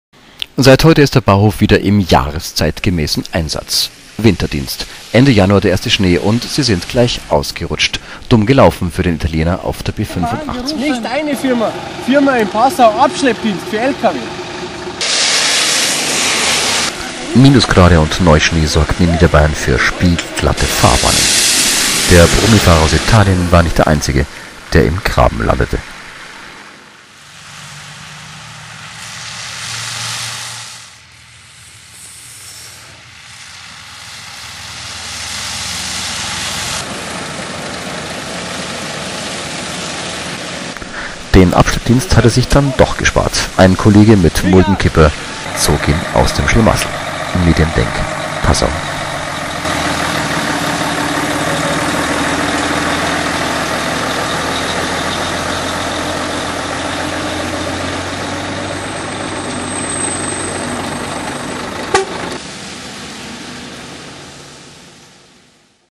Endlich Winter in Bayern
Brummifahrer auf der B 85 ausgerutscht